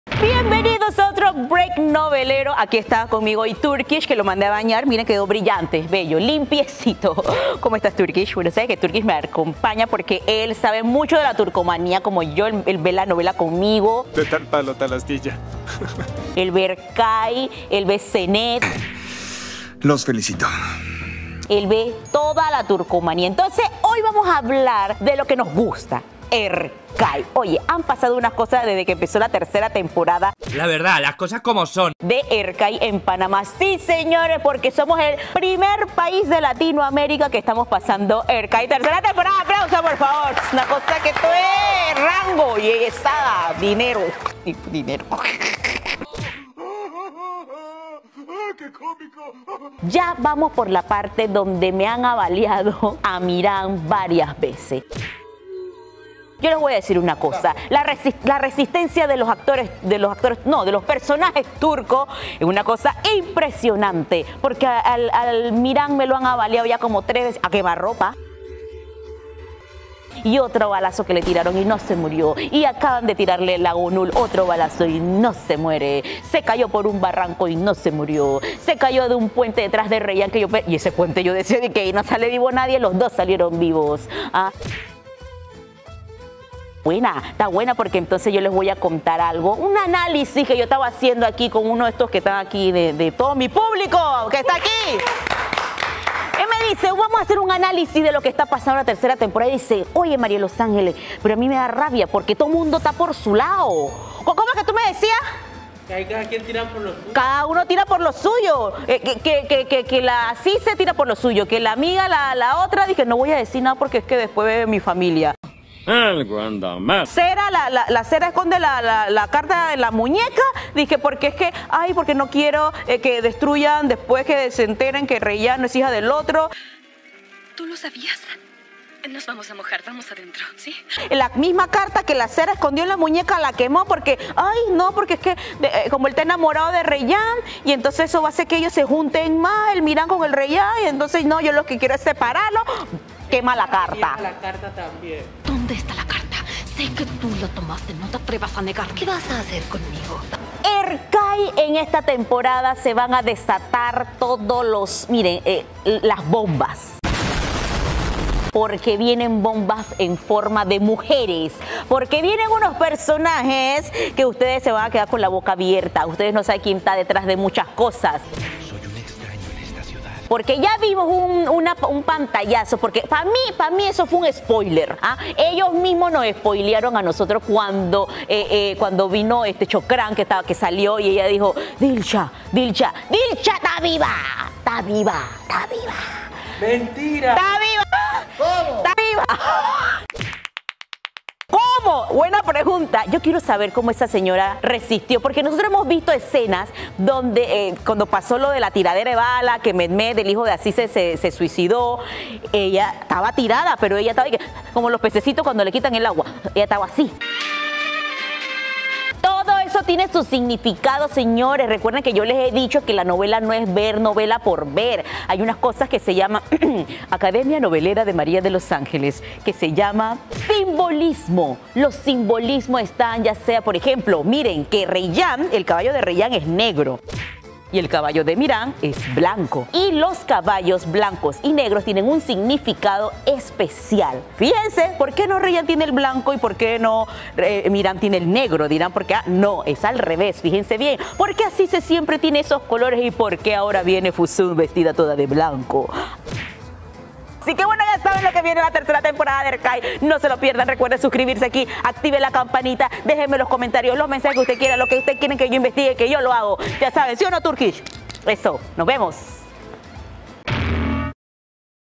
NOVELAS TM | ¿QUÉ ESTÁ PASANDO CON LA TERCERA TEMPORADA DE HERCAI?
PANAMÁ ES EL PRIMER PAÍS DE LATINOAMÉRICA EN TRASMITIR LA TERCERA TEMPORADA DE HERCAI.
ENTÉRATE LO QUE ESTÁ PASANDO EN ESTA TERCERA TEMPORADA A SUS PERSONAJES JUNTO A NUESTRA NOVELERA MARÍA DE LOS ANGELES.